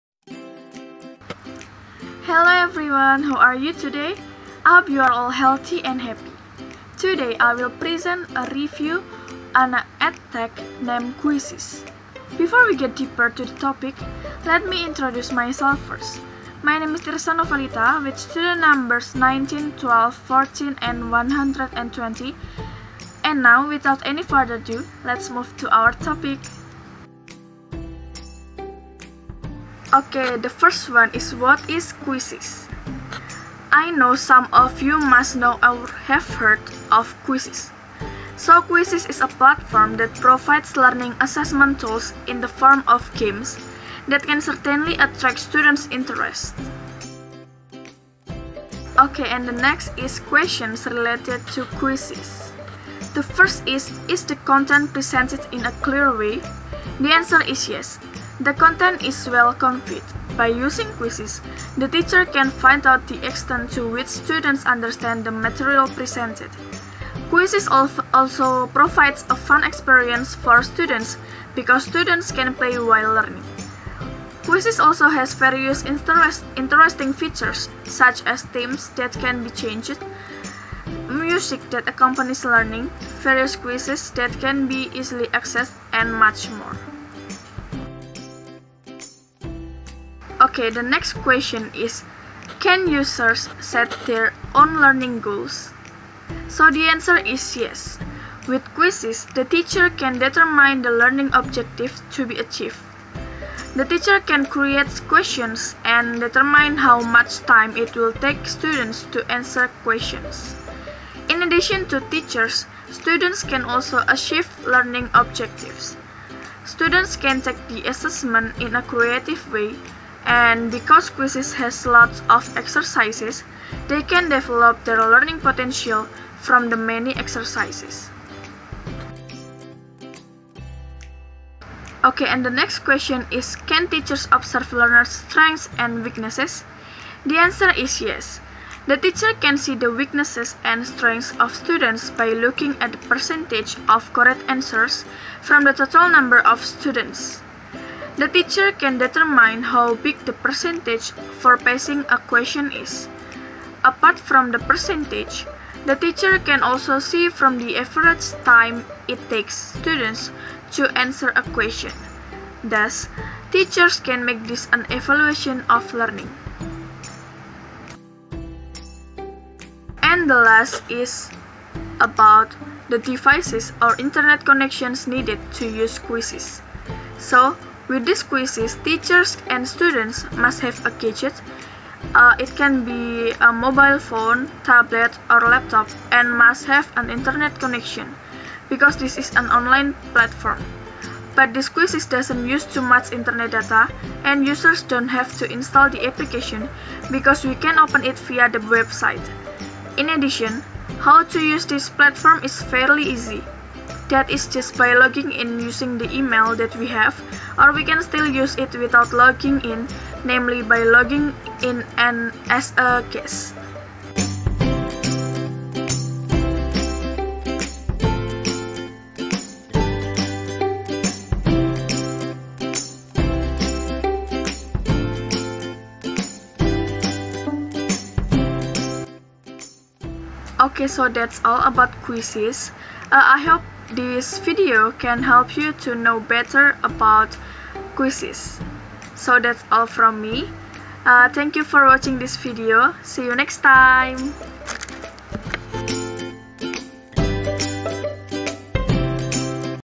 Review on EdTech: Quizizz!
Hello, everyone!!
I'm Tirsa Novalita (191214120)
This is my review about quizizz. I hope you will like it
Happy watching and thank you!!